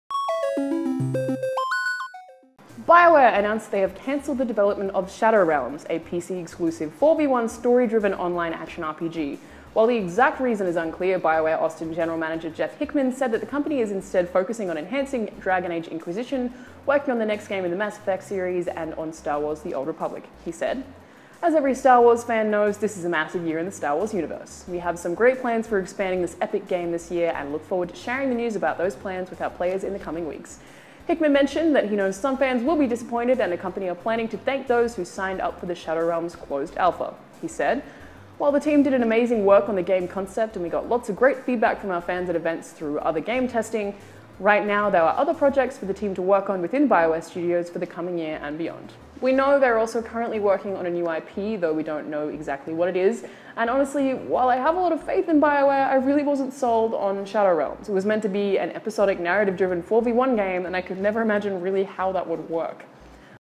BioWare Cancel Shadow Realms
BioWare announced that they have cancelled the development of Shadow Realms, a PC exclusive 4v1 story-driven online action RPG. While the exact reason is unclear, BioWare Austin general manager Jeff Hickman said that they company is instead focusing on enhancing Dragon Age: Inquisition, working on the next game in the Mass Effect series and on Star Wars: The Old Republic. He said:

“As every Star Wars™ fan knows, this is a massive year in the Star Wars universe. We have some great plans for expanding this epic game this year, and look forward to sharing the news about those plans with our players in the coming weeks.”

Hickman mentioned that he knows some fans will be disappointed, and the company are planning to thank those who signed up for the Shadow Realms Closed Alpha. He said:

“While the team did amazing work on the game concept and we got lots of great feedback from our fans at events and through other game testing, right now there are other projects for the team to work on within the BioWare studios for the coming year and beyond.”

We know they are also currently working on a new IP, though we know little about what that is, exactly. And, honestly, while I have a lot of faith in BioWare, I really wasn’t sold on Shadow Realms – it was meant to be an episodic, narrative-driven 4v1 game, and I could never imagine how that would actually work.